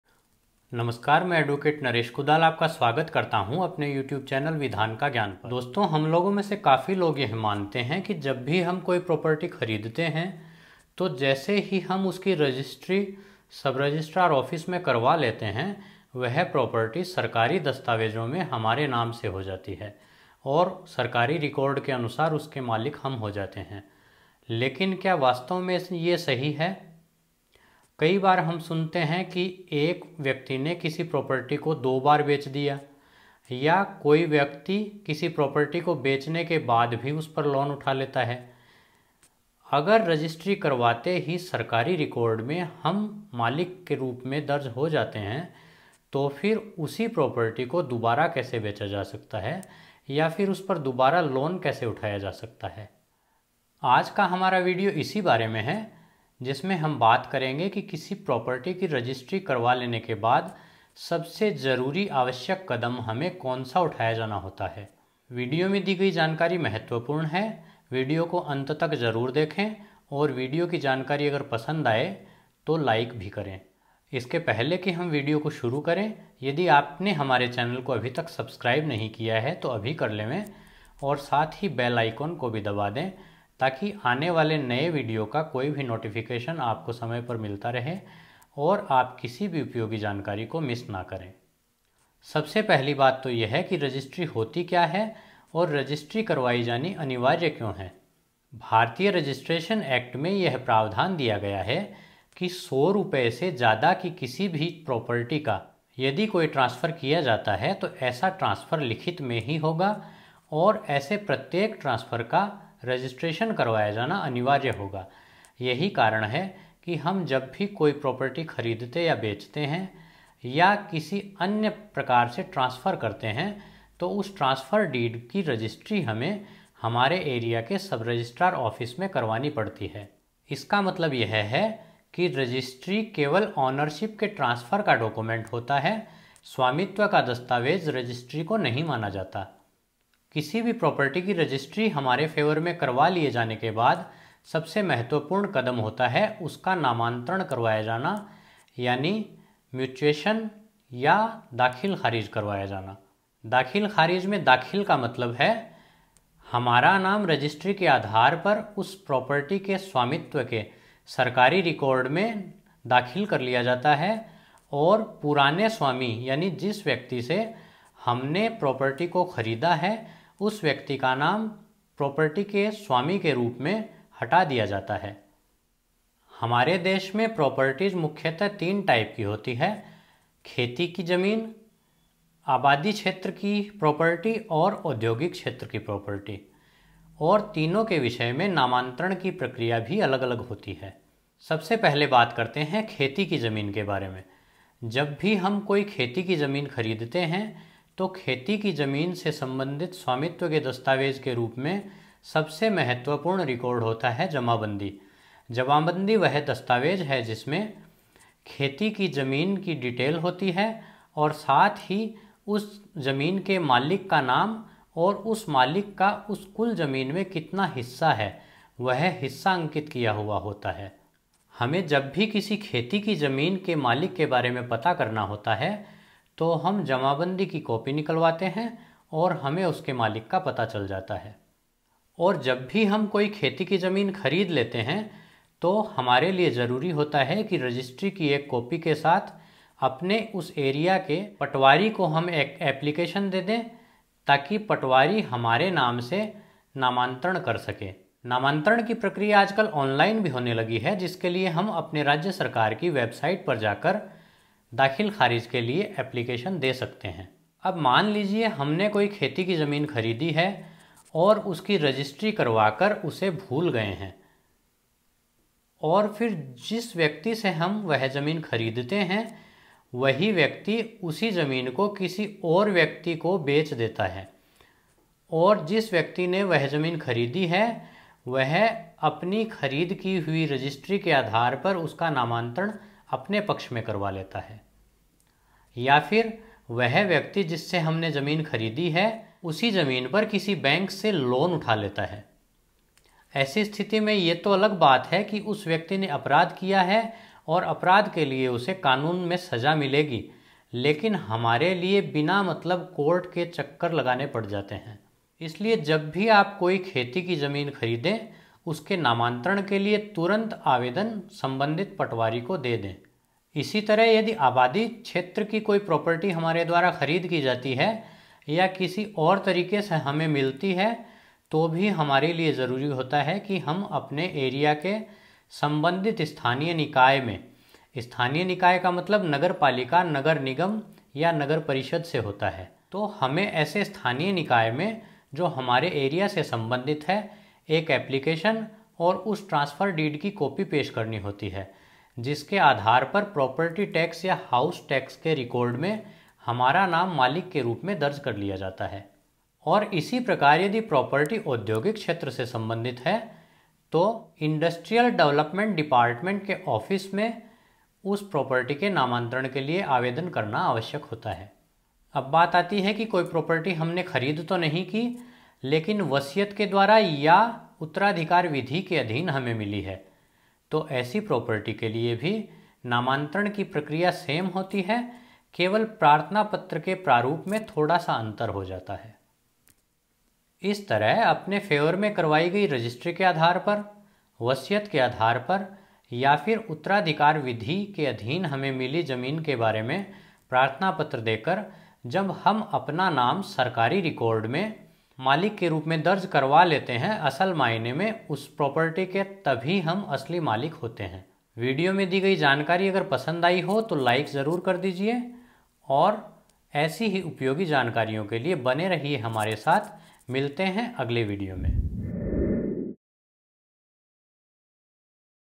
Mutation of Land and Property, Dakhil Kharij Kya Hai, Property Registry (23)
Consultation starts just from 499/- Only

Mutation of Property, Dakhil kharij kya hai, vidhan ka gyan
Mutation of Land and Property dakhil kharij online property law mutation online apply

In this video it is stated that when we register a property when we purchase it, the property does not become our name as soon as we register it, but it has to be registered in our name, then our name in the official record of that property belongs to the owner Can be recorded in the form.

Thanks,
Team Naresh Kudal Advocate,
Vidhan ka Gyan